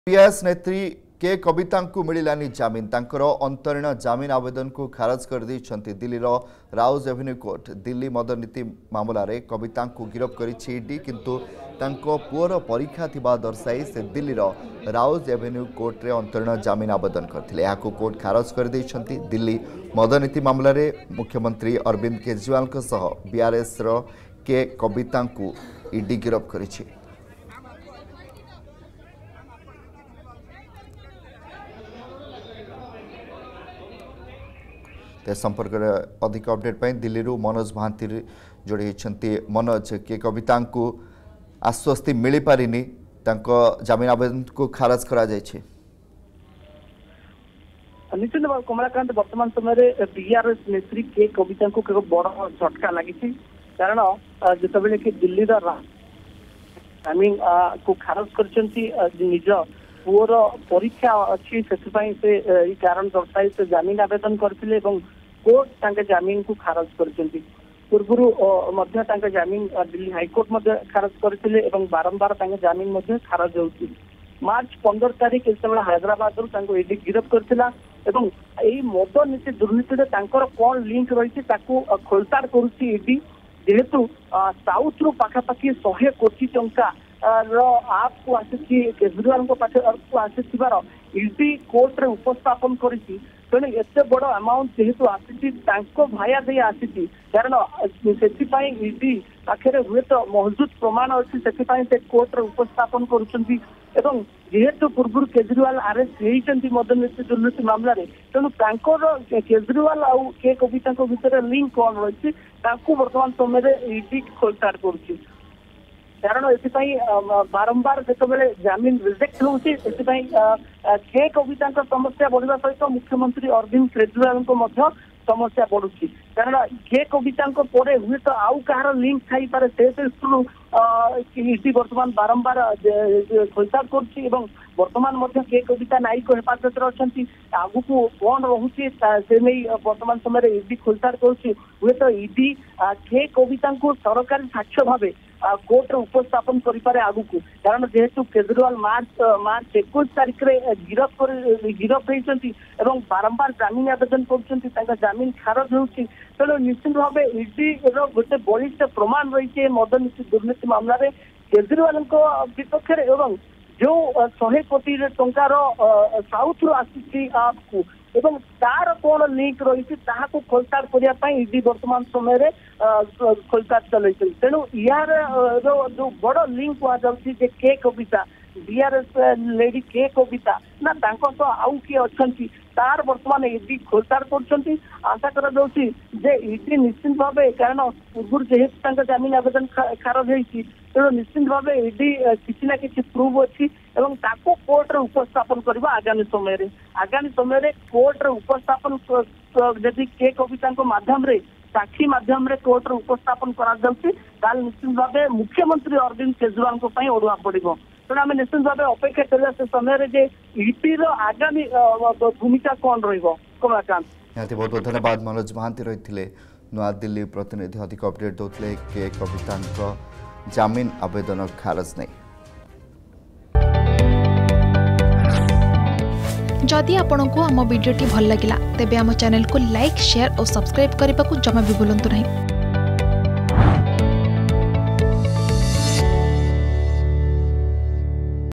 ବିଆରଏସ୍ ନେତ୍ରୀ କେ. କବିତାଙ୍କୁ ମିଳିଲାନି ଜାମିନ୍ || K Kavitha || Excise Policy Case
ବିଆରଏସ୍ ନେତ୍ରୀ କେ. କବିତାଙ୍କୁ ମିଳିଲାନି ଜାମିନ୍ ।